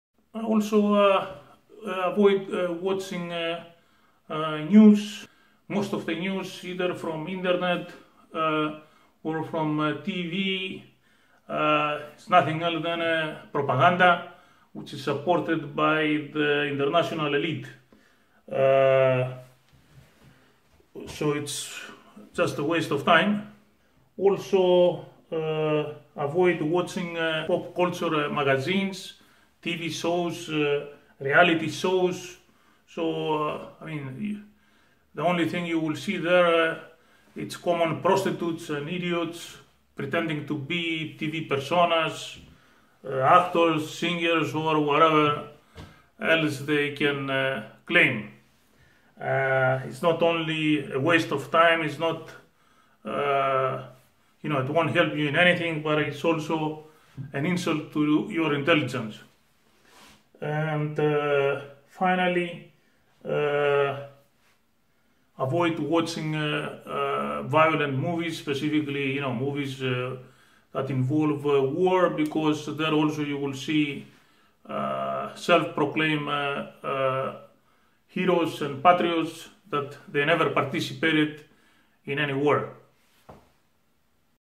Healthy student lifestyle helps academic achievement (mental health, supplementary 2/2)
George Lerakis  continues with a couple of more tips regarding the mental health of students. He recommends that students shouldn't waste time watching news on TV or internet  because the majority of news stations these days are just propaganda machines mostly owned by the international elite.
Also he strongly suggests students to avoid watching all these pop culture magazines reality shows where you see common prostitutes self proclaimed actresses, tv personas, singers etc, and idiots claiming to be comedians. Is not only a waste of your time is bad for your academic achievement and insult to your intelligence Lerakis claims.
Finally he recommends students to avoid watching violent movies and more specifically war movies where extremely well paid actors  playing hero and patriot roles but in real life they never been in a single war.